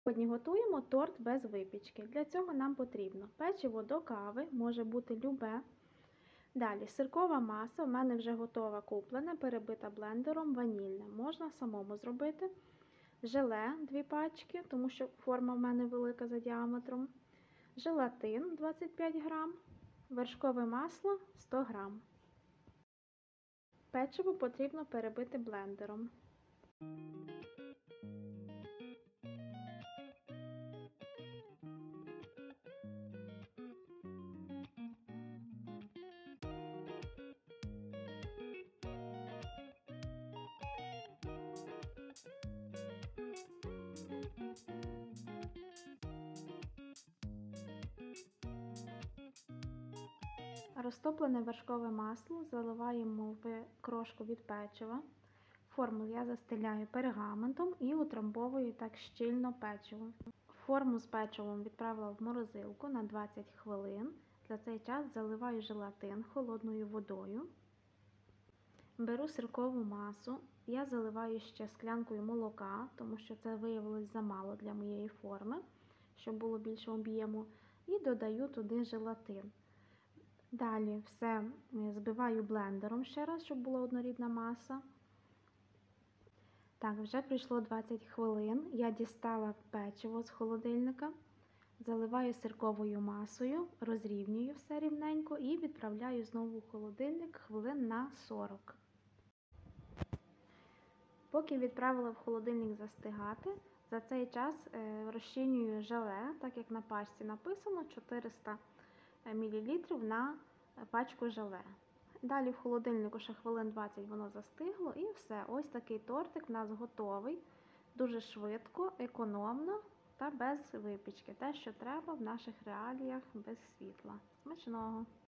🍰Торт без випічки ✅Економний торт (коли немає світла а хочеться солодкого)
Швидкий та бюджетний🍰 Торт без випічки 🍰
•печиво
•сиркова маса (сир-творог збити блендером)
•желатин 25гр
•желе 2пачки
•масло вершкове 100гр.